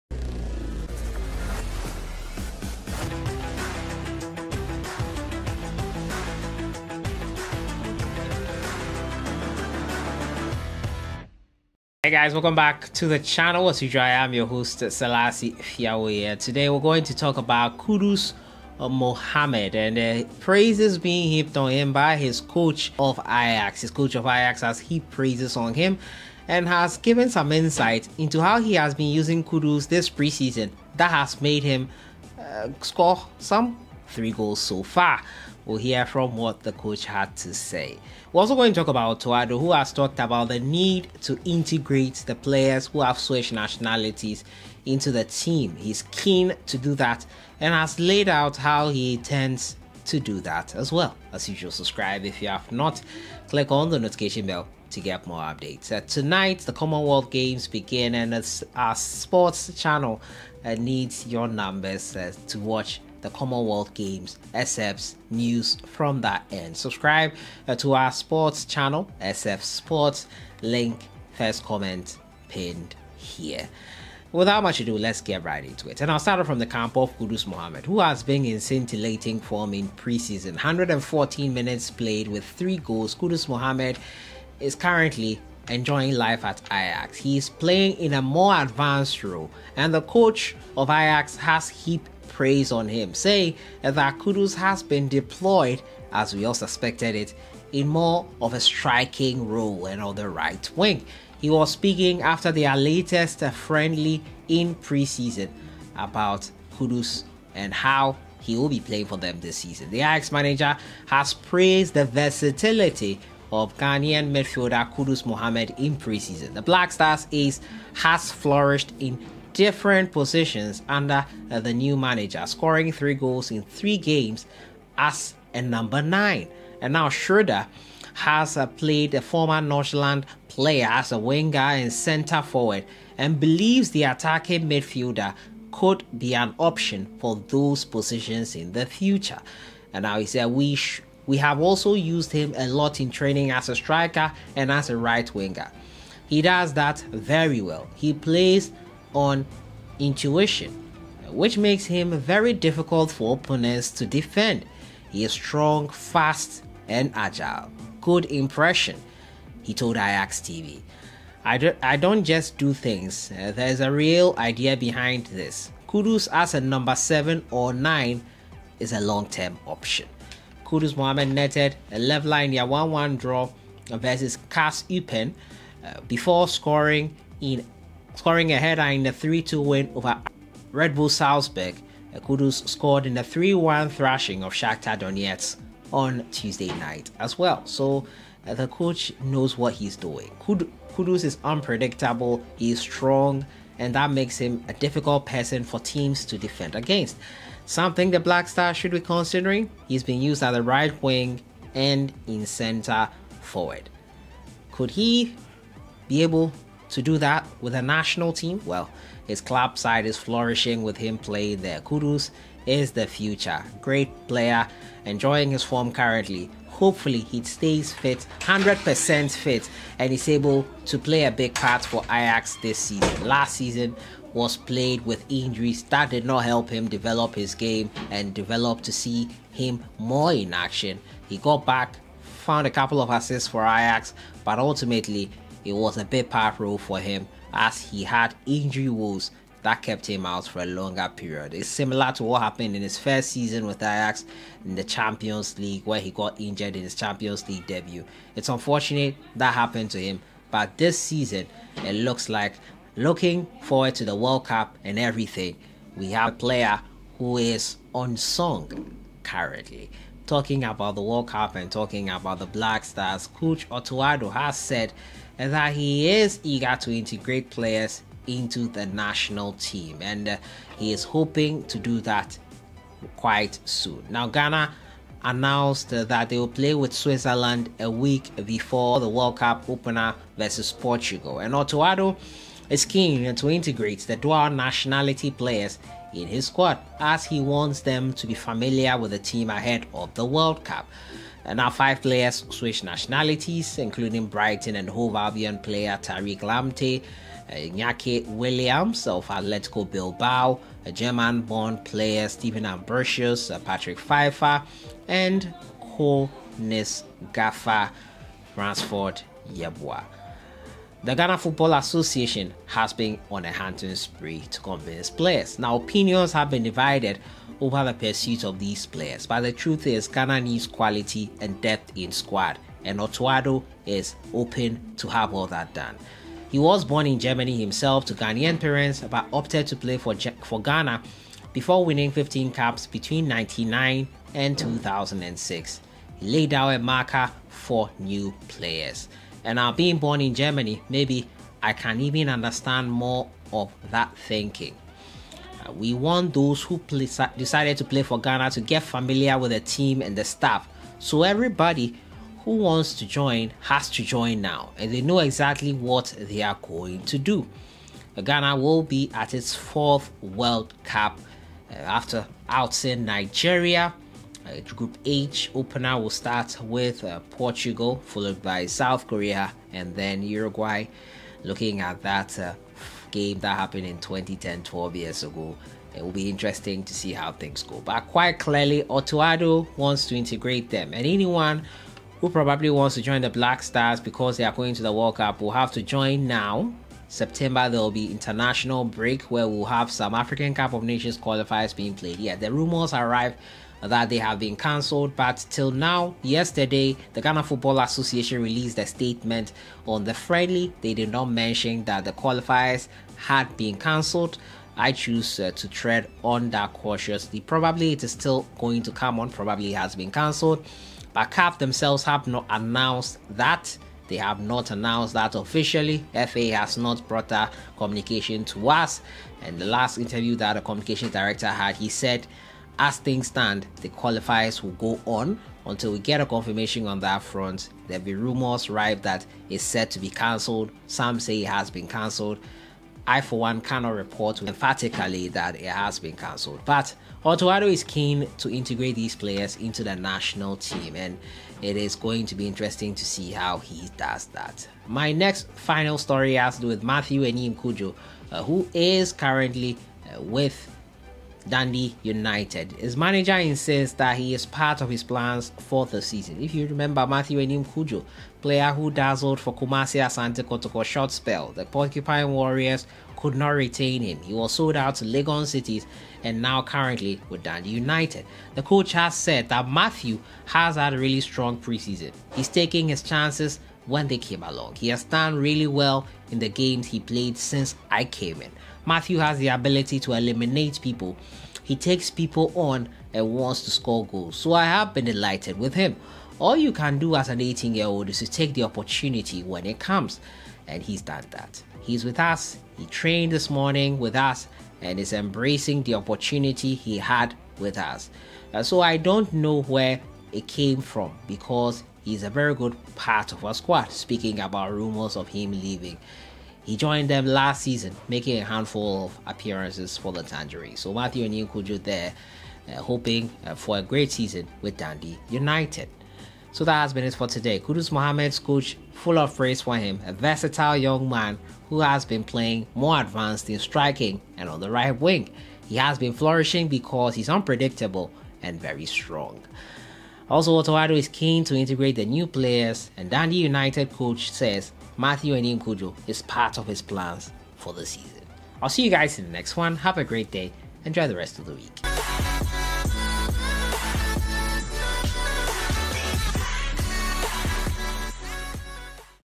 KUDUS MOHAMMED'S VERSATILITY PRAISED BY AJAX COACH & OTTO ADDO KEEN TO INTEGRATE PLAYERS
BLACK STARS PLAYER KUDUS MOHAMMED HAS BEEN PRAISED FOR HIS VERSATILITY & OTTO ADDO KEEN TO INTEGRATE NEW PLAYERS INTO THE BLACK STARS FOLD